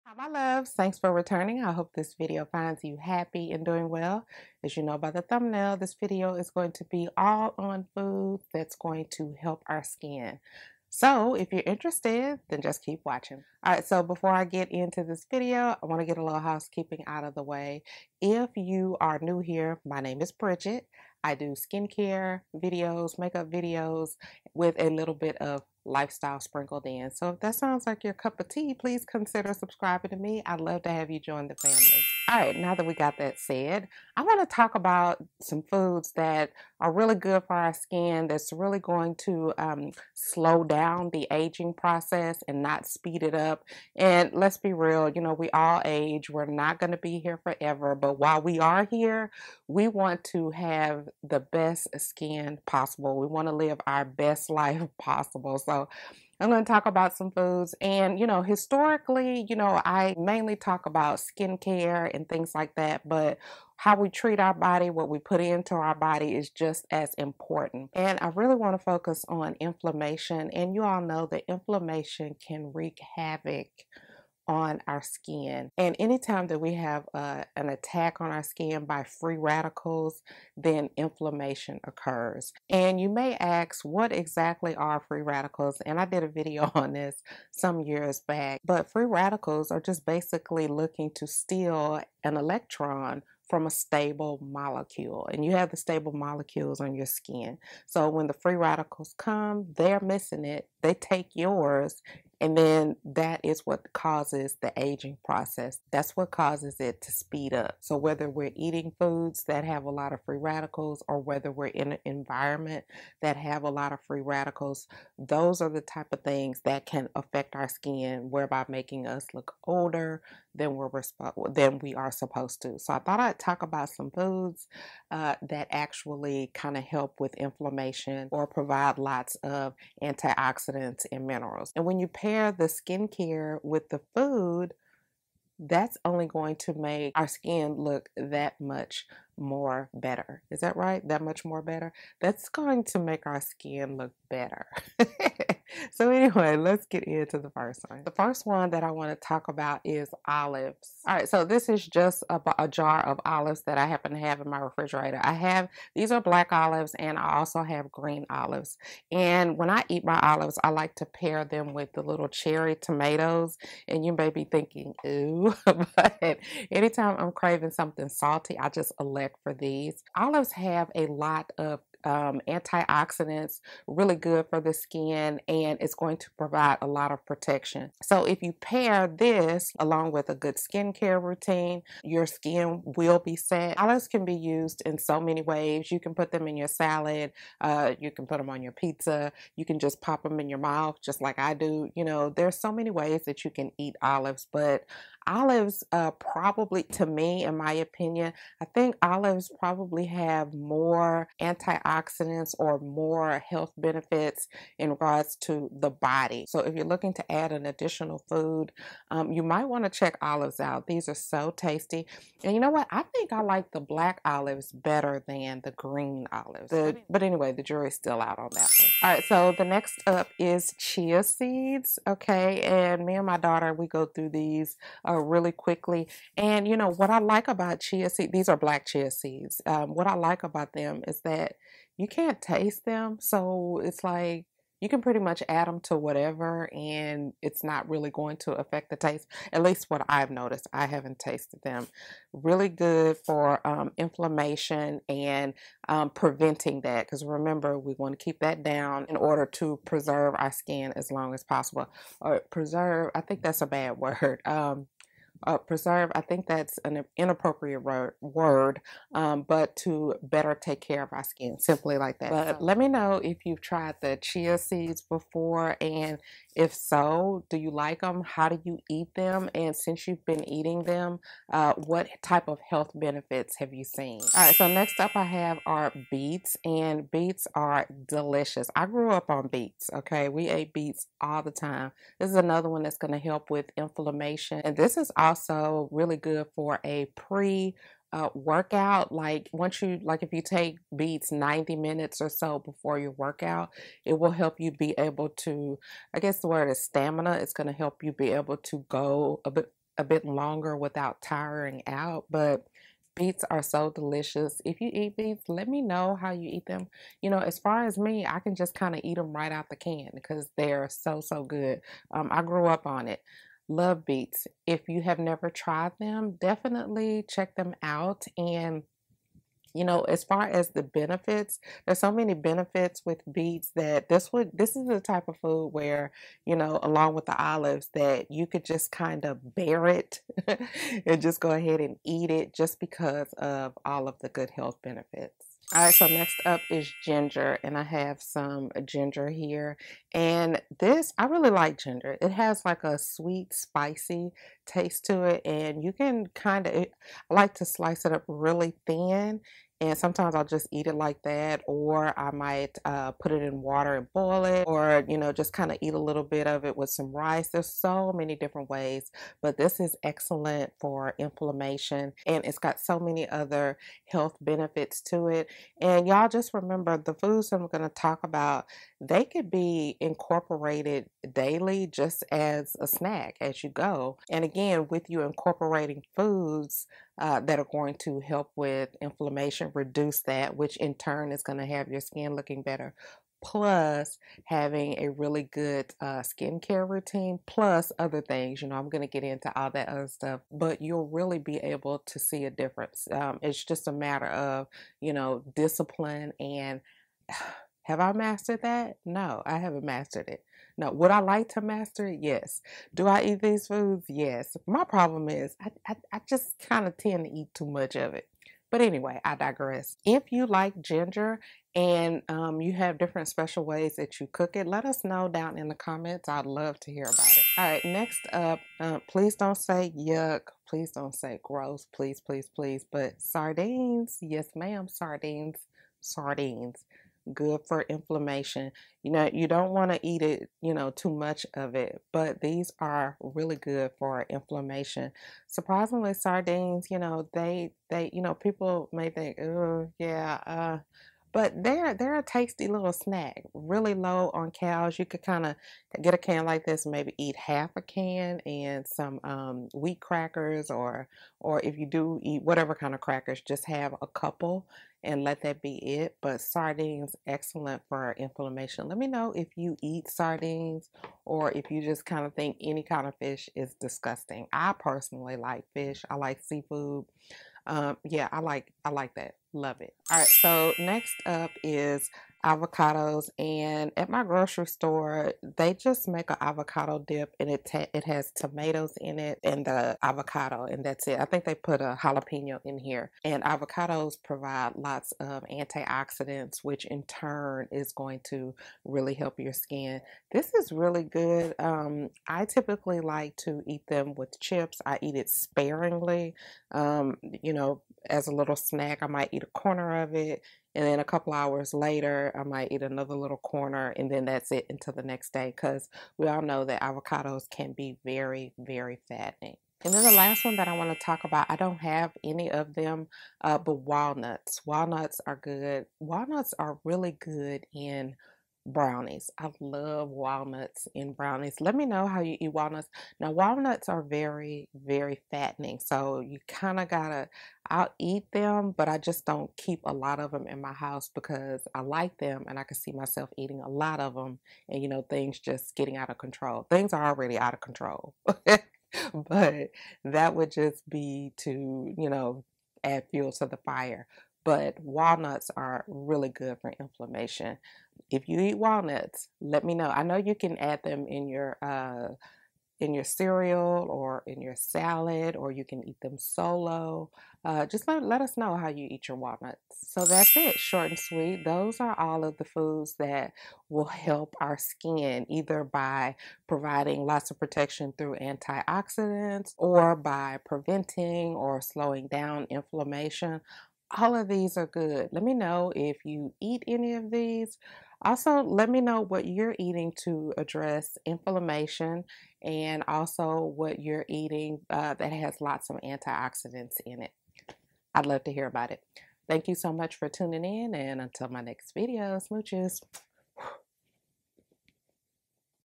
Favorite Foods for the Skin ☆ Address Signs of Aging Easily
Hi everyone! Today I want to talk about a few of my favorite foods that will help with the aging process. Please let me know if you have tried any of these foods and if so how well they worked for you.

What's on my face
PYT BEAUTY | One & Done Setting Spray
CHARLOTTE TILBURY | Smokey Eyes Are Forever Kit plus a POP red loose pigment
Other videos that you may find helpful on your journey to beautiful healthy skin: